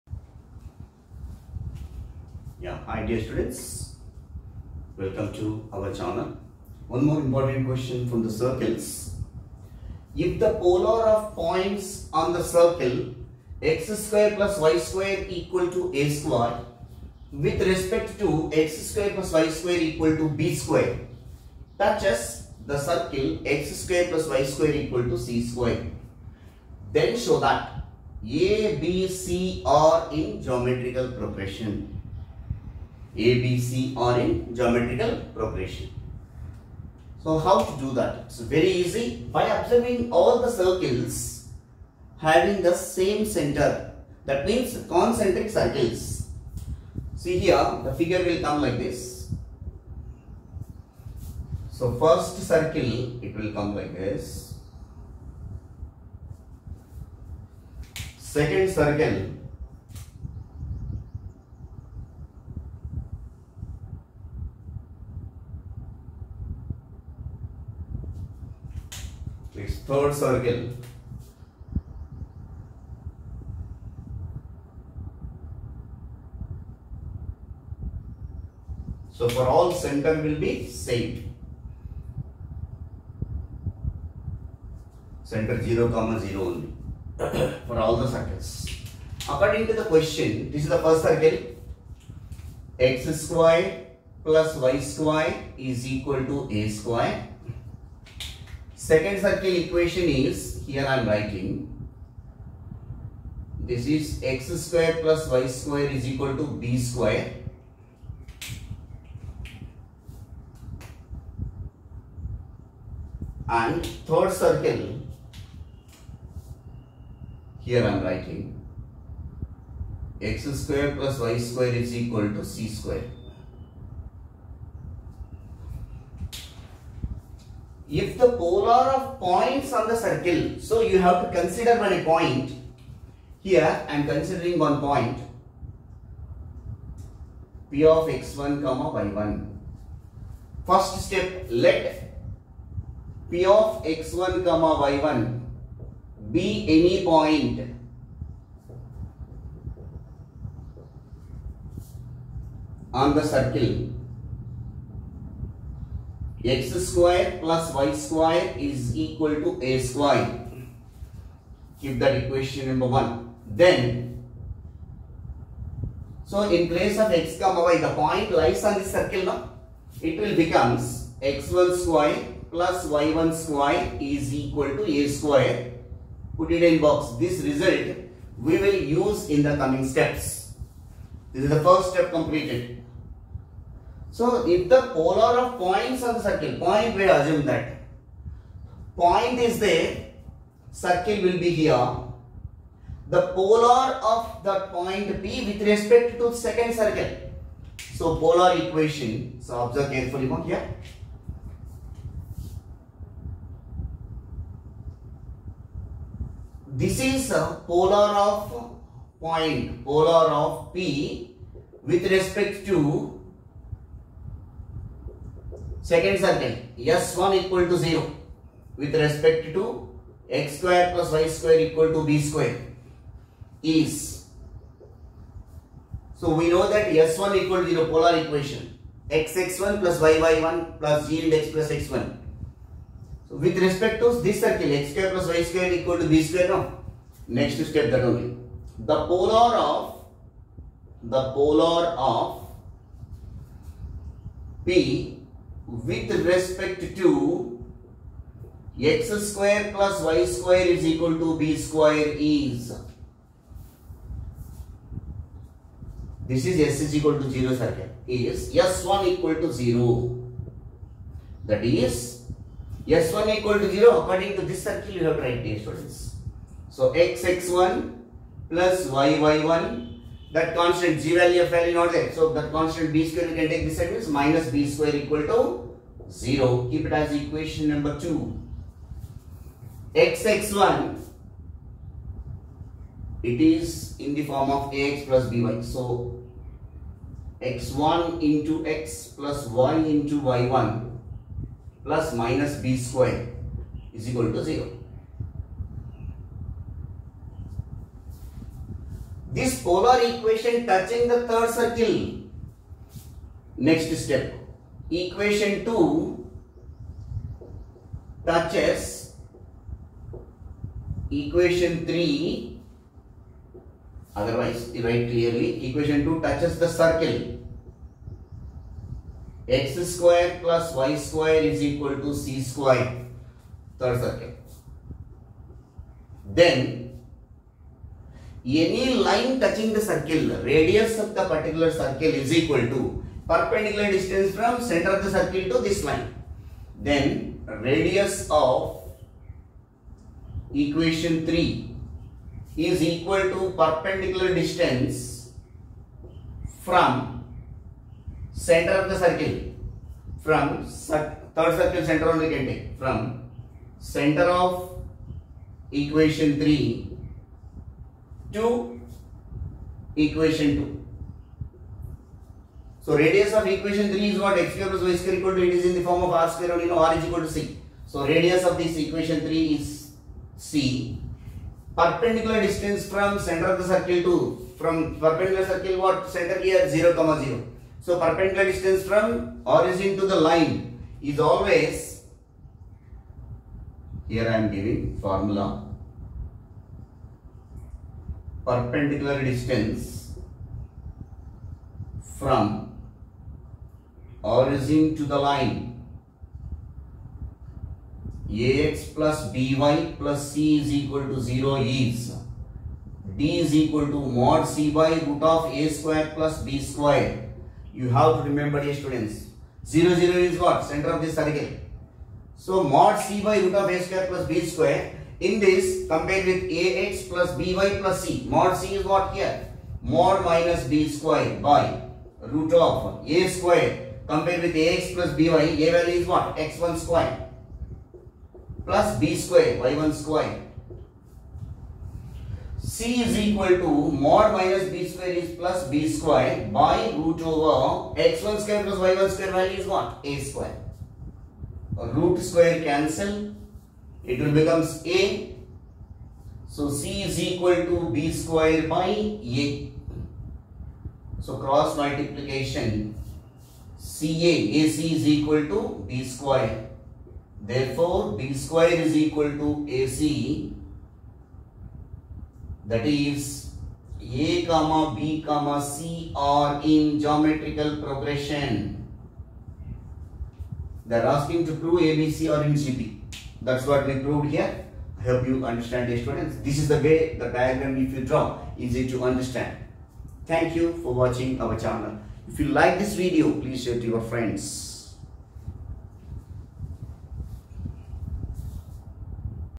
if the polar of the points on the circle x²+y²=a² w.r.t x²+y²=b² touches x²+y²=c² thena,b,c in G.P
if the polar of the points on the circle x²+y²=a² w.r.t x²+y²=b²  touches x²+y²=c² then prove that a,b,c are in geometrical progression